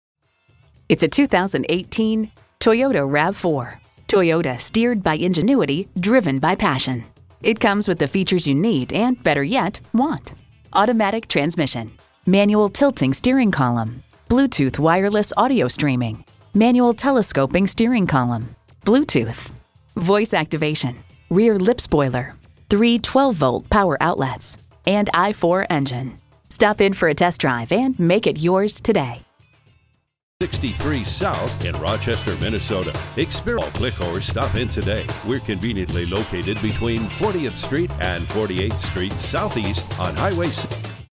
New 2018 Toyota RAV4 Rochester, MN S42488 - SOLD
Year: 2018
Make: Toyota
Model: RAV4
Trim: LE
Engine: 2.5L 4-Cylinder DOHC Dual VVT-i
Transmission: 6-Speed Automatic
Color: Gray
Interior: Fb20
Stock #: S42488
VIN: JTMBFREV2JJ226522
2018 Toyota RAV4 4D Sport Utility 2.5L 4-Cylinder DOHC Dual VVT-i AWD LE Magnetic Gray Metallic New Price! Recent Arrival! We look forward to earning your business.

Address:
4365 Canal Place Southeast